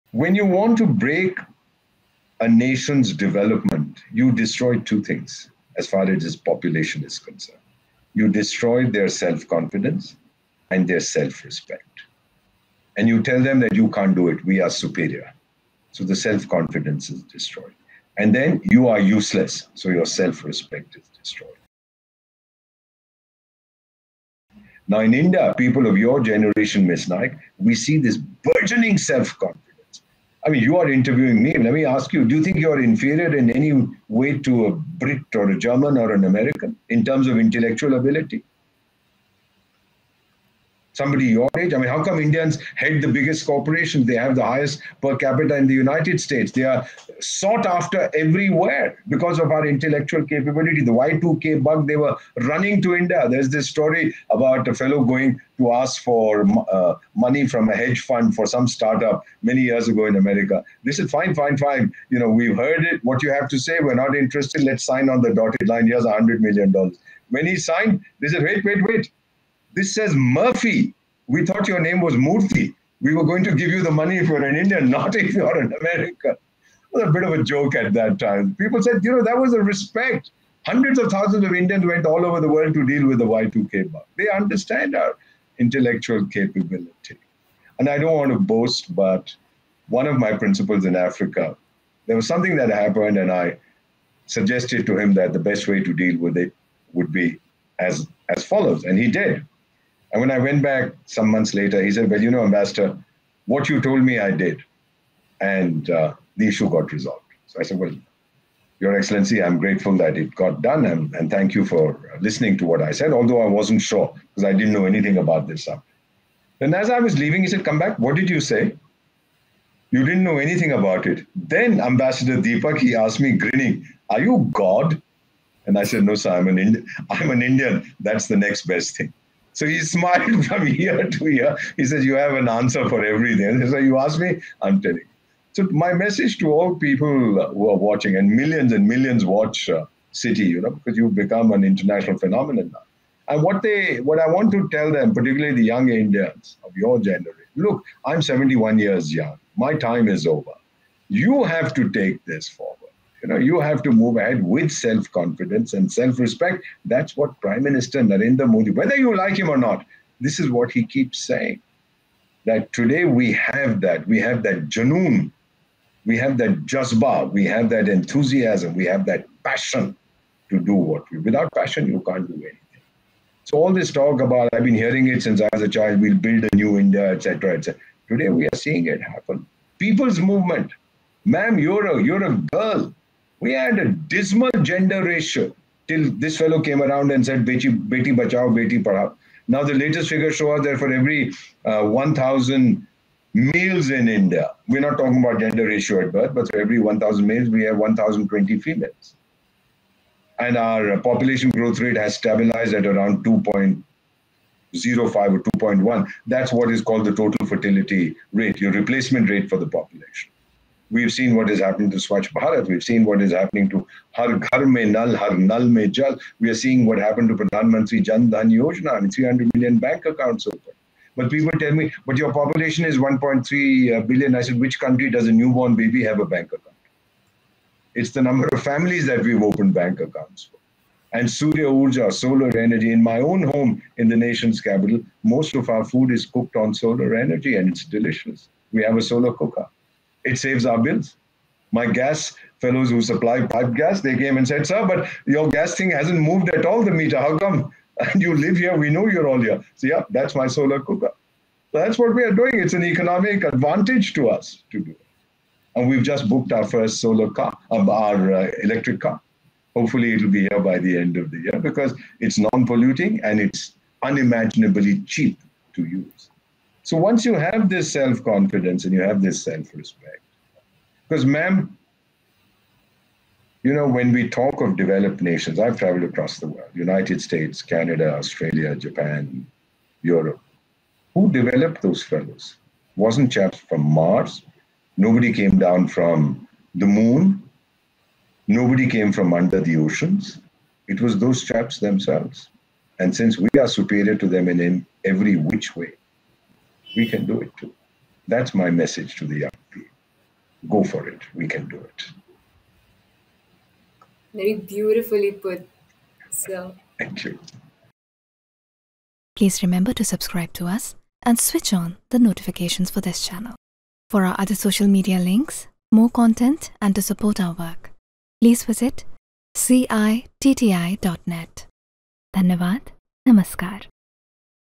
"Go for it, India - we can do it."
"There is nothing that Indians cannot acheive if we put our minds to it." If you have children, ask them to ignore the naysayers & watch Ambassador Deepak Vohra's inspirational invocation to Indian youth